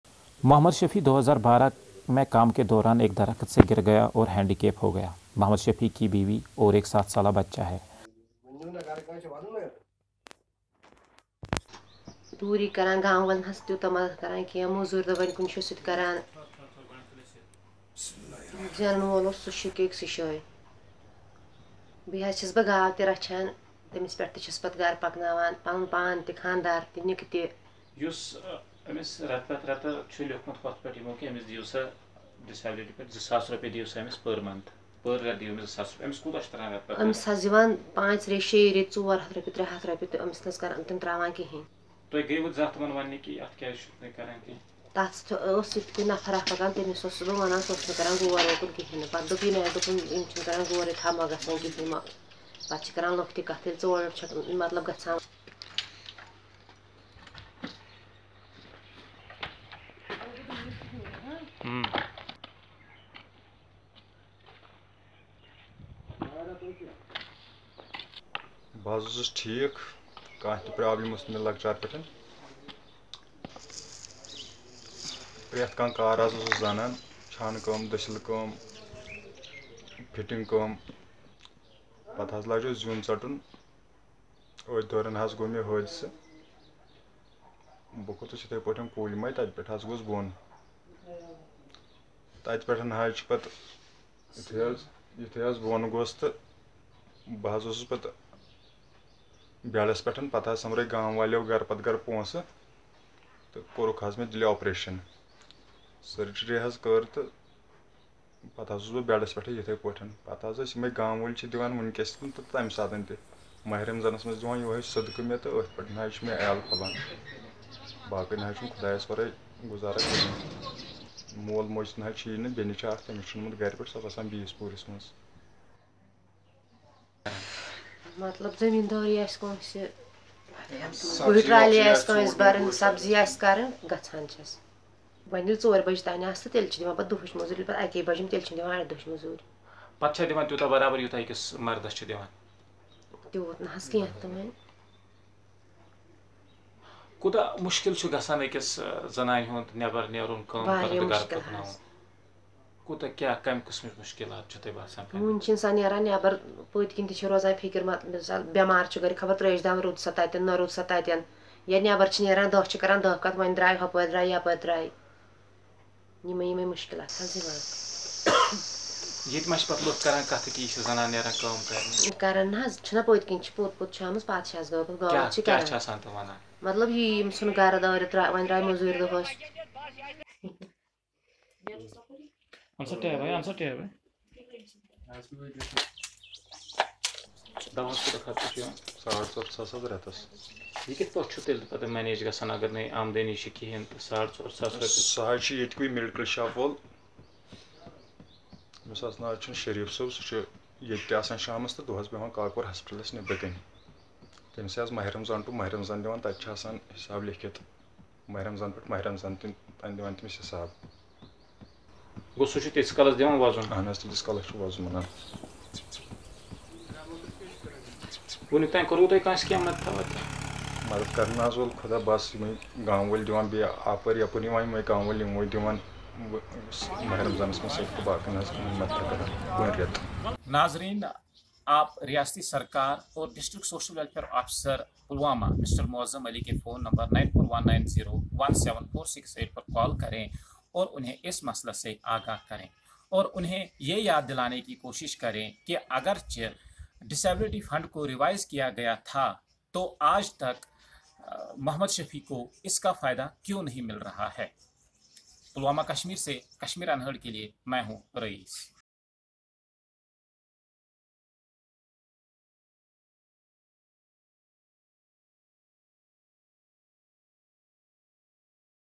'Revised Disability Pension Scheme' Not Benefiting 100% Disabled Mohammad Shafi | Rayees Reports
Mohammad Shafi Dar of Maanavji Pulwama was declared 100% disabled by the health department after he fell from a tree while working. Her wife Meema now works in the fields of neighbors to earn livelihood for her and her family. Meema, is paid very less wages as compared to her male fellows. And her husband Mohammad Shafi gets an amount of Rupees 400 only by the Social Welfare Department every month as assistance under Indra Gandhi National Disability Pension Scheme while he is eligible for an amount of INR 2000 as per his disability certificate provided by Doctors.